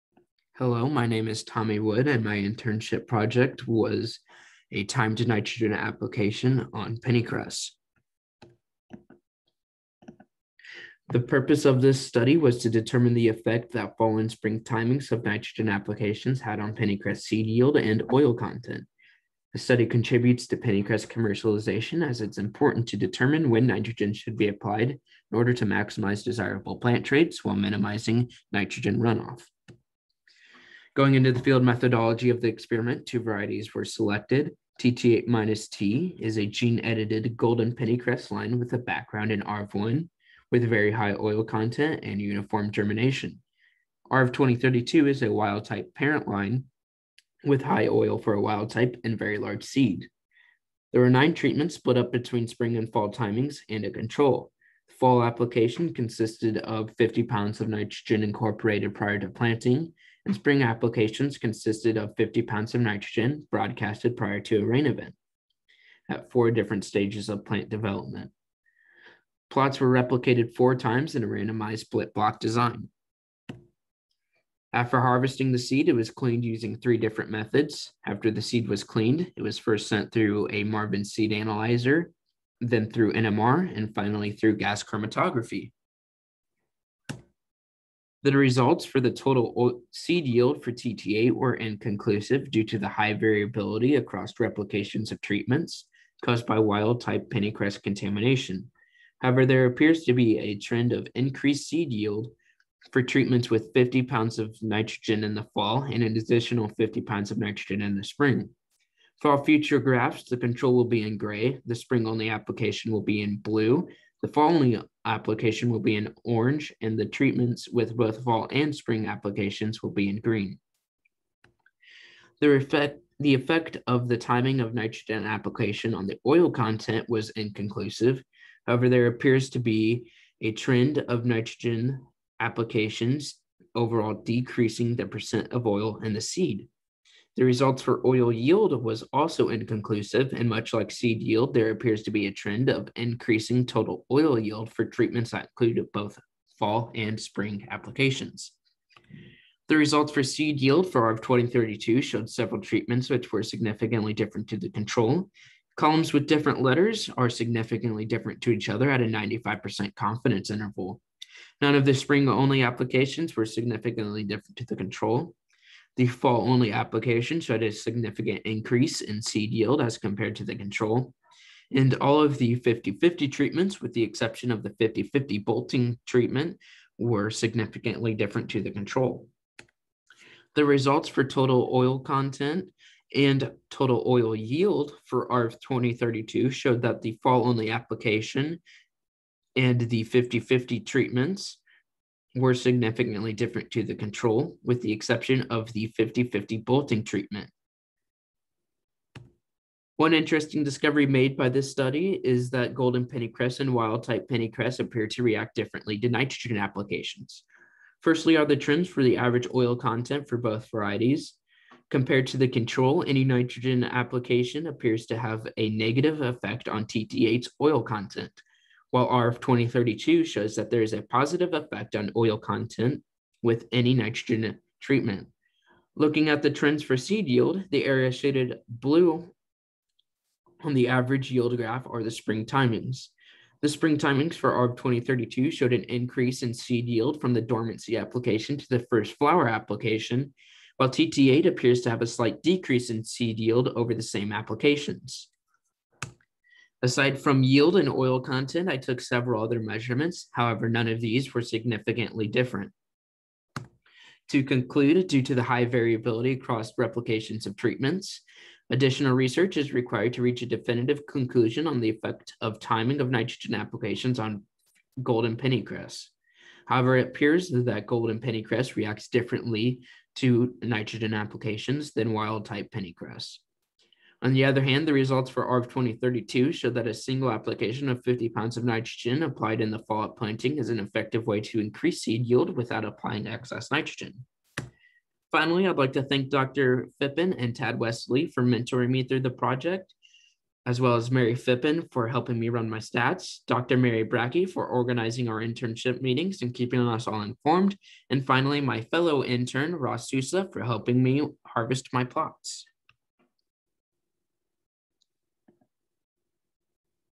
Timed Nitrogen Application
Tommy Wood (Western Illinois University / Agricultural Science) worked with IPREFER Project Director Win Phippen to improve the quality and seed yield of pennycress in the spring with minimal nitrogen application.

For information on IPREFER (Integrated Pennycress Research Enabling Farm & Energy Resilience) please